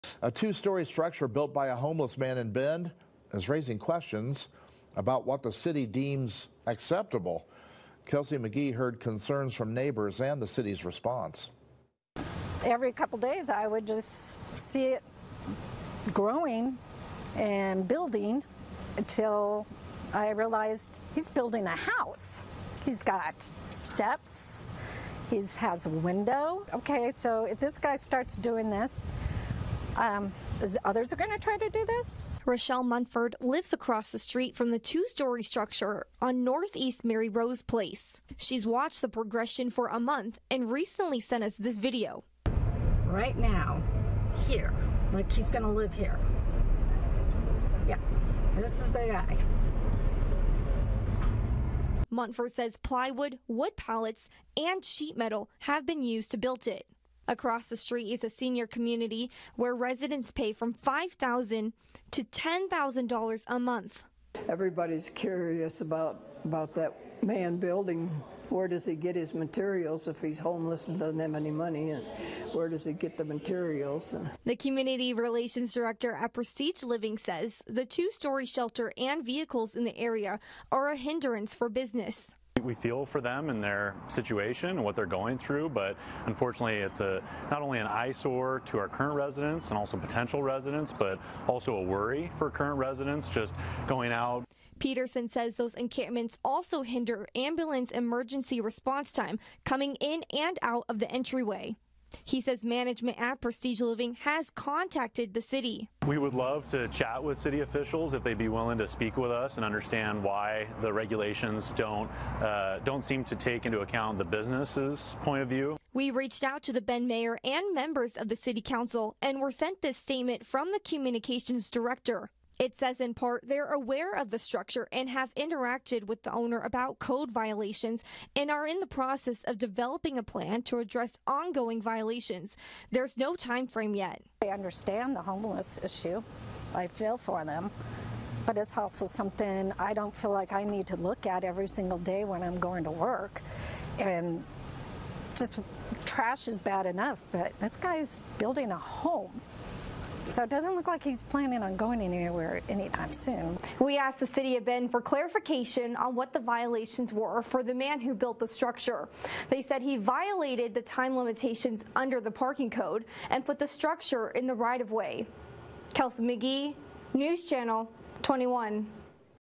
Two-story structure built on wheels along NE Bend street sparks concern over what city allows ...
A two-story mobile structure built by a homeless man along a busy NE Bend street is raising questions about what the city deems acceptable.